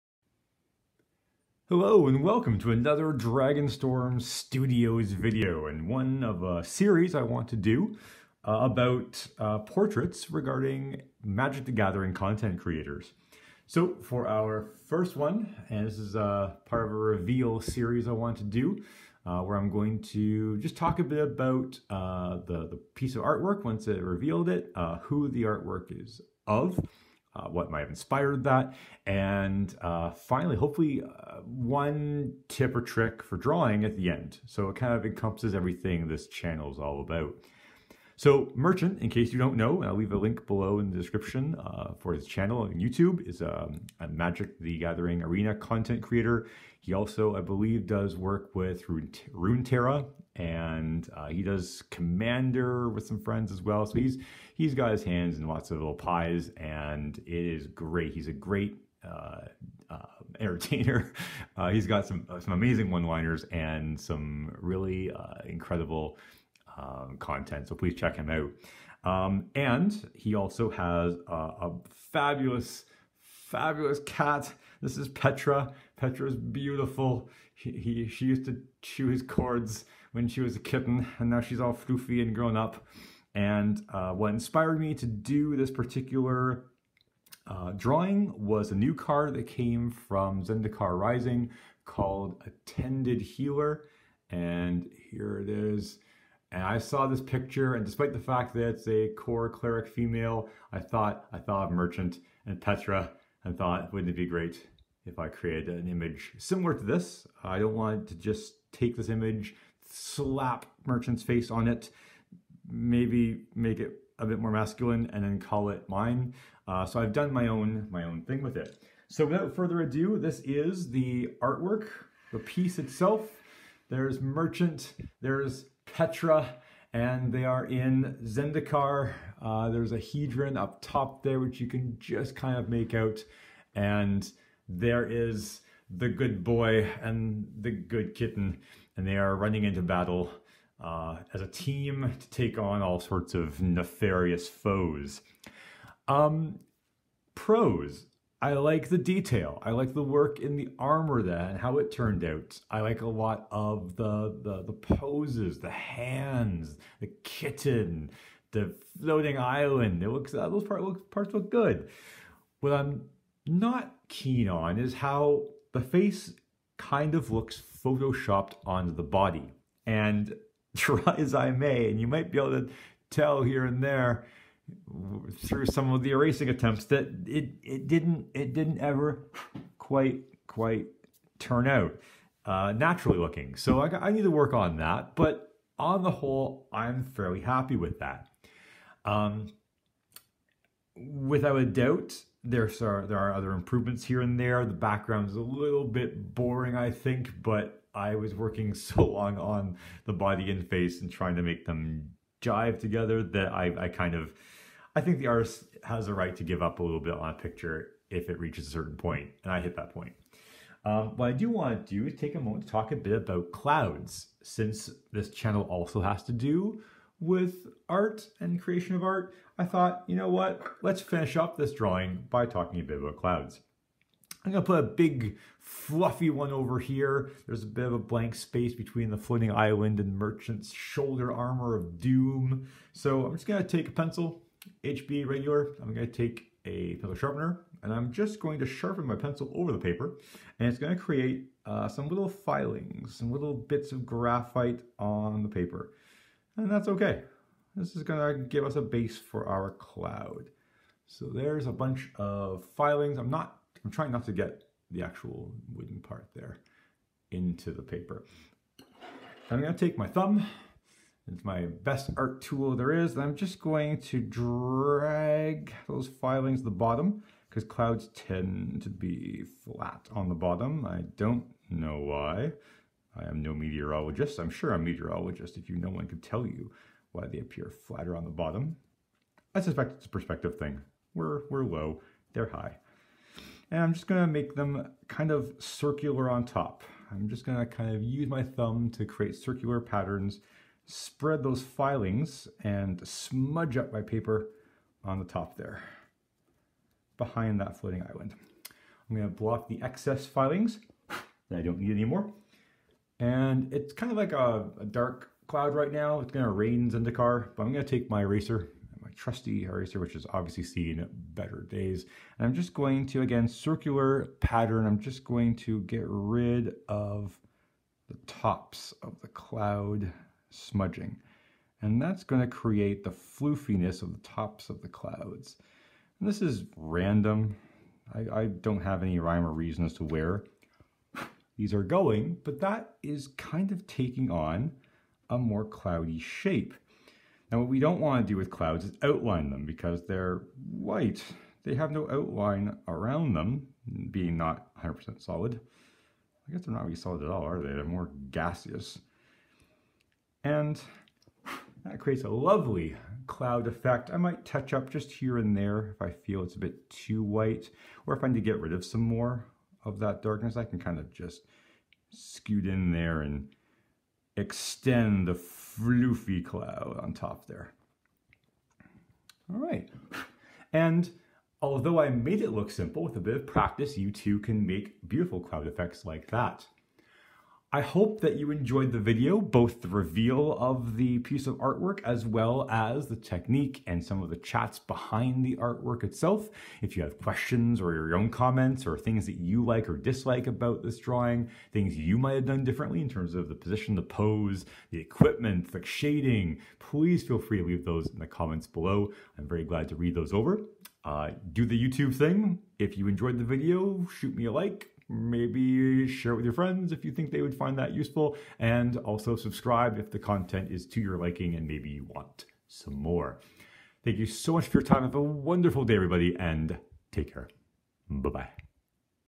Content Creator Portrait: Merchant Drawing Reveal
A video where I reveal and chat about the drawing of Merchant from Magic the Gathering, Runeterra, and various other content fame on Youtube. Leader of the No-Pants Gang, caretaker of Petra the cat, and overall great Youtuber!
I will also demonstrate drawing clouds. =)